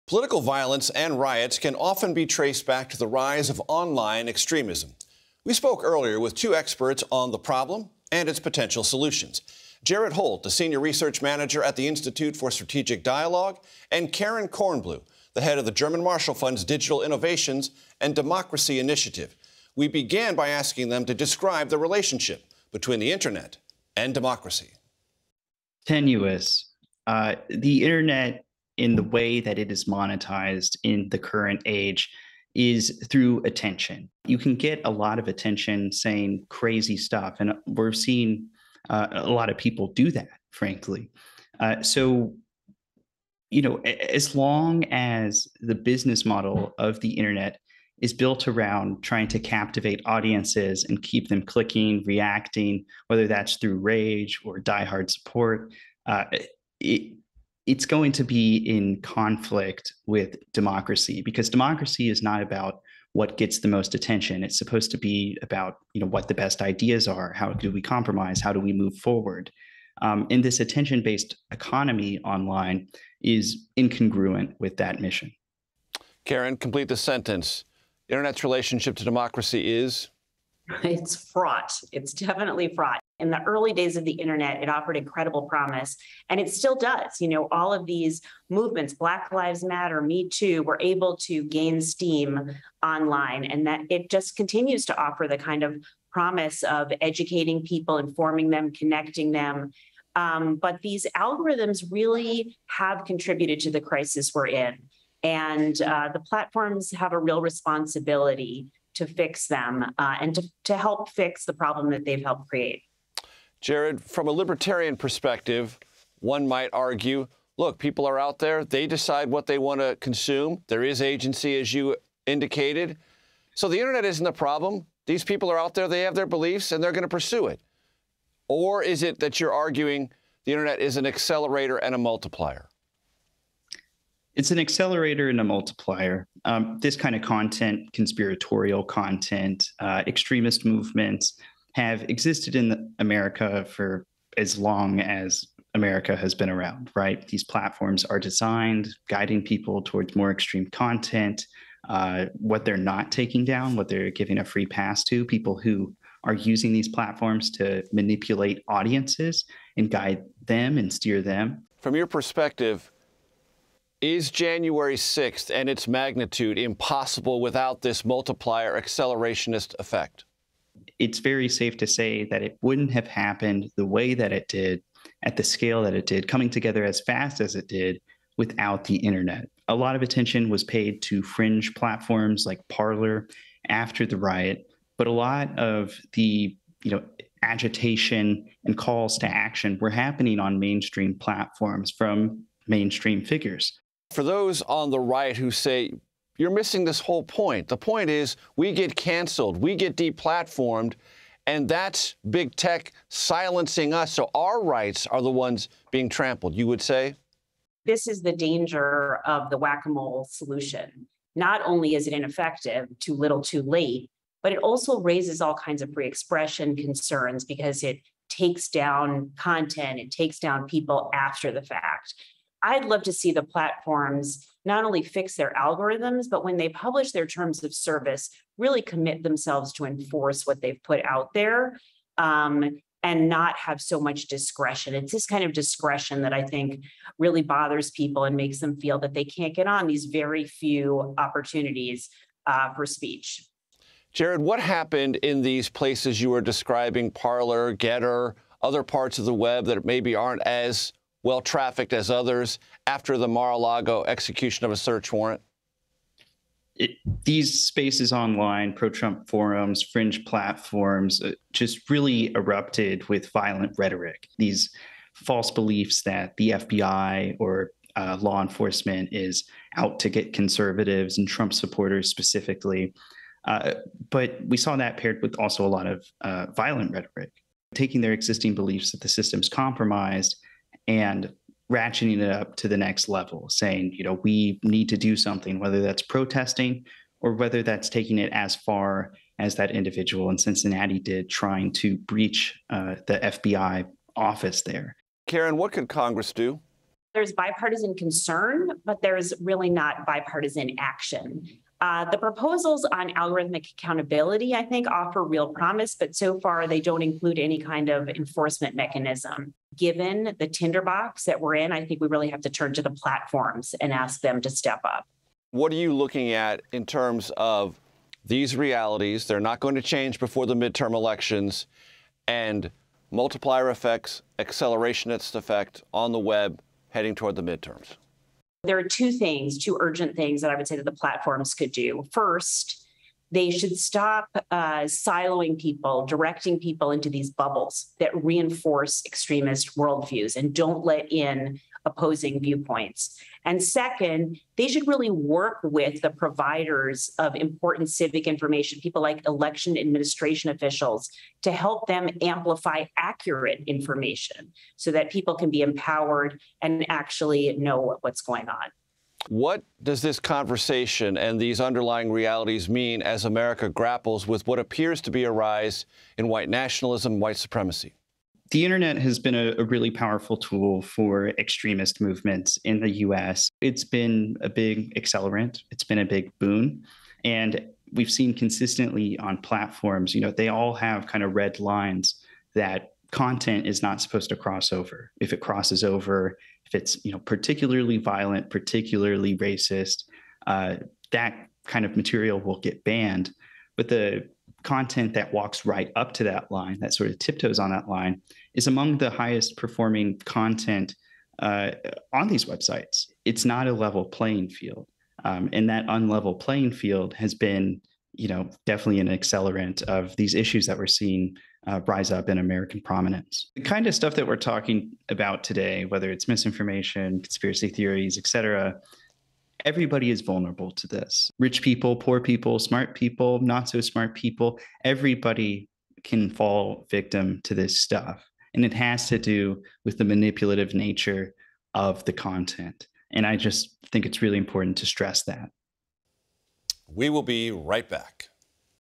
Tracing the links between the internet and extremism
Jared Holt of the Institute for Strategic Dialogue and Karen Kornbluh of the German Marshall Fund discuss the internet's impact on democracy and the rise of online extremism.